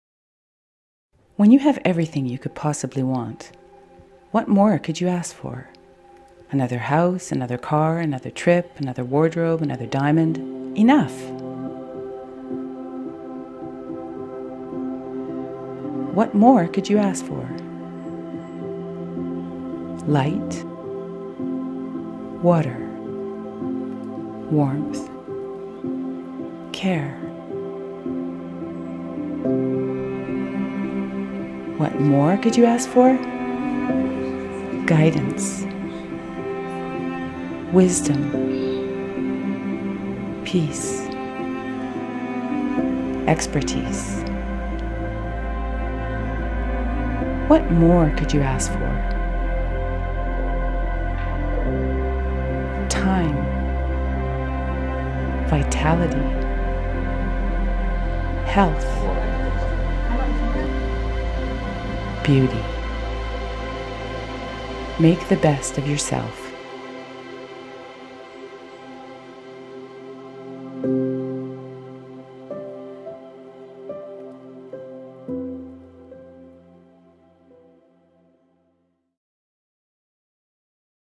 ¿Qué más se puede pedir? - Clinique La Prairie
Recupera tu belleza y tu luminosidad interior con las últimas y más eficaces tecnologías no invasivas. Recupera el control de tu salud, tu verdadera riqueza. Descubre todos nuestros tratamientos de medicina estética, bienestar y longevidad.